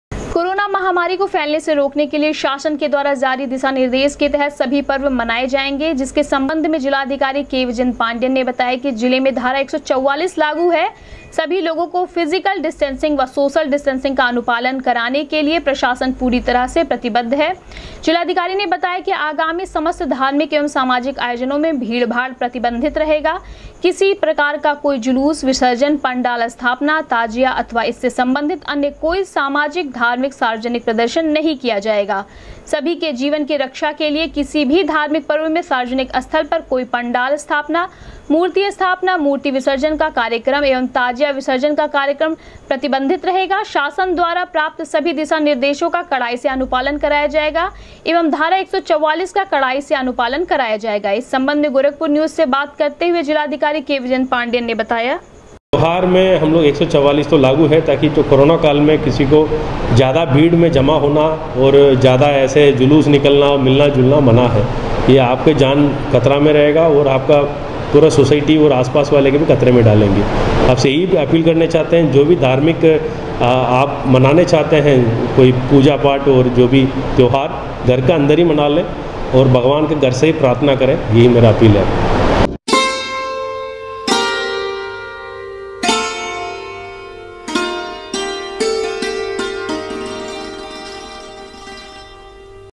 गोरखपुर न्यूज से बात करते हुए जिलाधिकारी ने कहा,त्यौहारों के मद्देनजर जिले में लागू है धारा 144
गोरखपुर न्यूज से बात करते हुए जिलाधिकारी ने कहा,त्यौहारों के मद्देनजर जिले में लागू है धारा 144, कोरोना महामारी की वजह से किसी भी धार्मिक आयोजनों में नही है भीड़ भाड़ की इजाजत, मूर्ति स्थापना, ताजिया स्थापना सहित अन्य कार्यक्रमो पर रहेगा प्रतिबंध